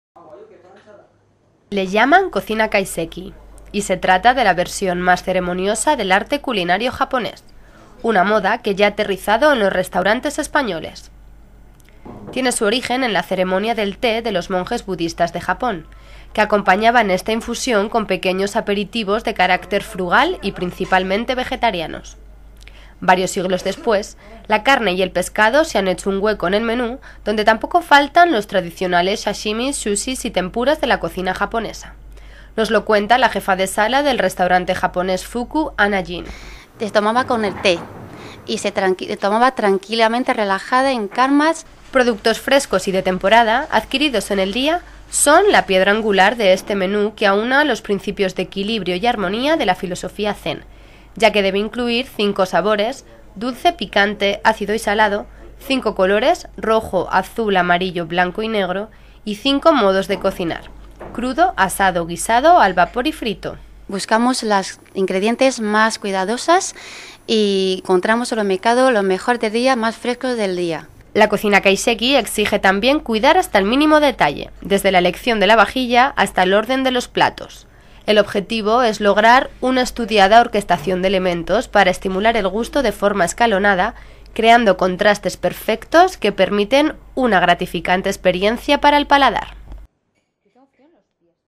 Cocina kaiseki: tradición budista y gastronomía nipona
La tradición budista y los mejores y más frescos productos japoneses de temporada se unen en la cocina kaiseki, la versión más sofisticada y ceremoniosa del arte culinario nipón, originaria de la época medieval y que se ha convertido ahora en una moda gastronómica que se abre hueco en España.